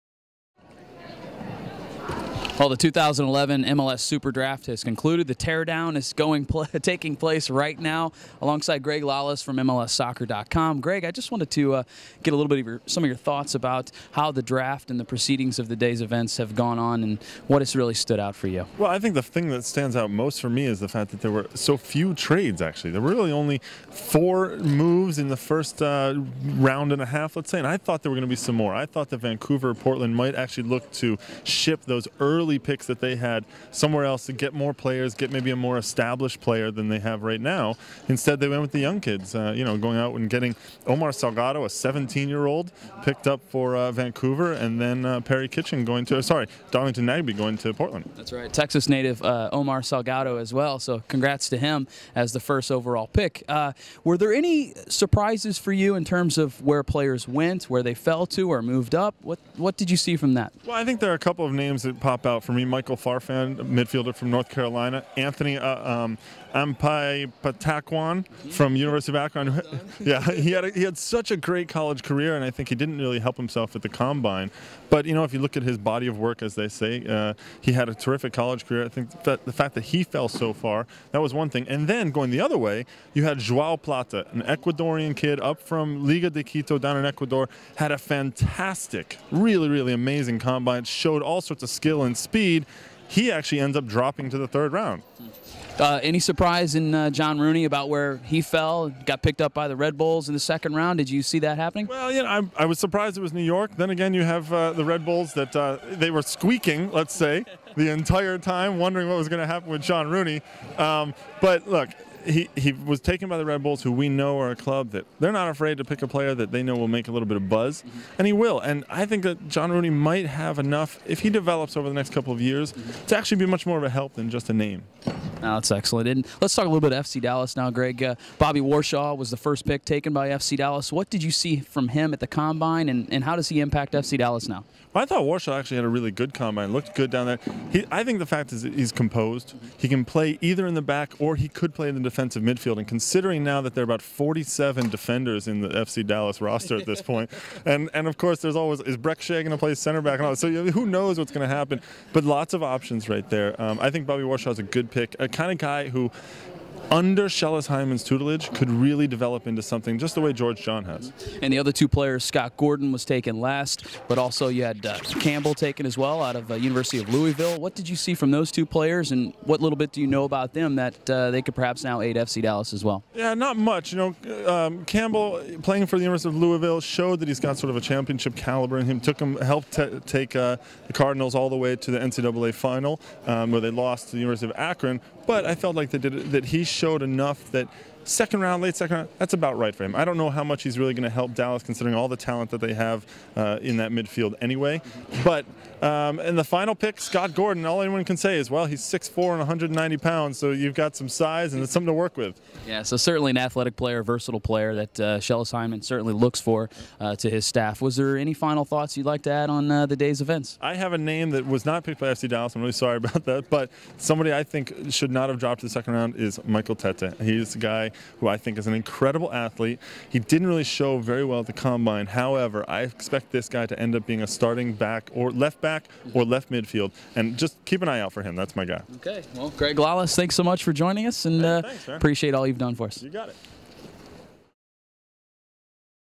SuperDraft: Greg Lalas
Bobby Rhine spoke to Greg Lalas of MLSsoccer.com about the FCD draft picks.